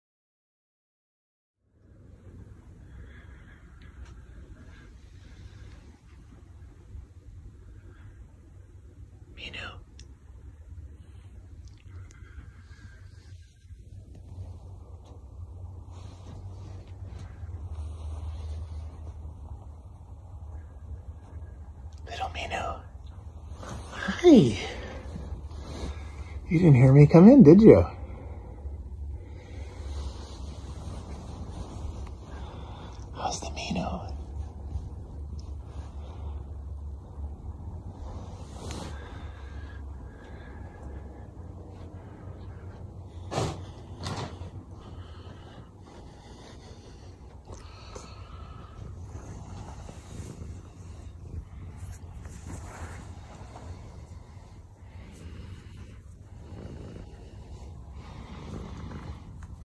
Surprising Little Minou On The Big Bed | Shorts From Milwaukee Life
This short is footage of the late great Sassy cat (aka Minou), back when she was mobile enough to get up on the bed by herself, snoozing in our big bed while I sneak up on her and surprise her. She slept most nights curled up with me on that side of the bed, and spent a good portion of her day there after we got up for the day's work

If you'd like to see more videos with Sassy as well as her dog and cat siblings, you might check out this playlist